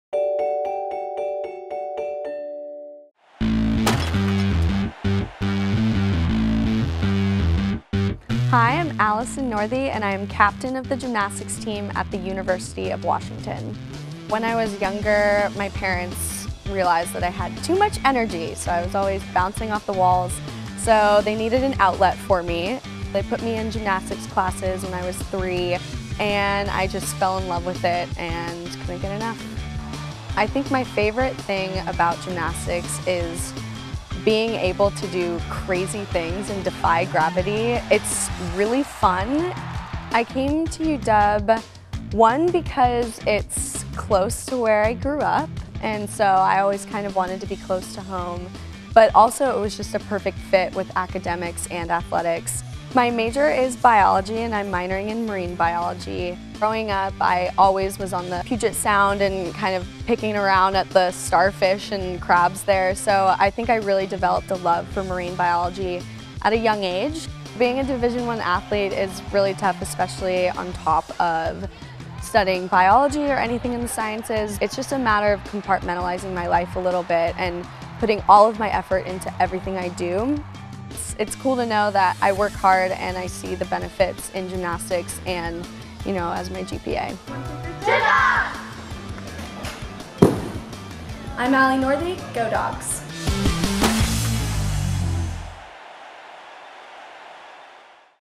UW - The Gold Standard - Allison Northey
Profile of UW student athlete Allison Northey, UW Women’s Gymnastics.

06/30/16